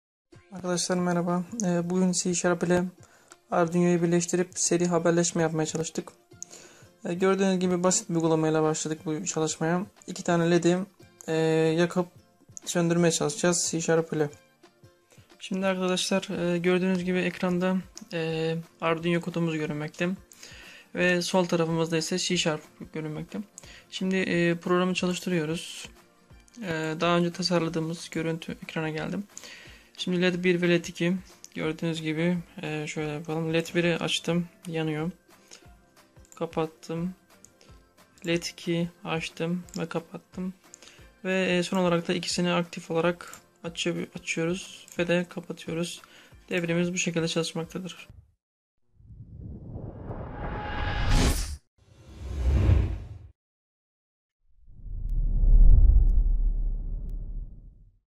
Arduino\C# ile Seri Haberleşme
Arduino ile haberleşen arayüzler tasarlamak ve bu arayüzlerden fiziksel dünya ile etkileşim kurmak en çok aranılan konulardan biri haline gelmiştir.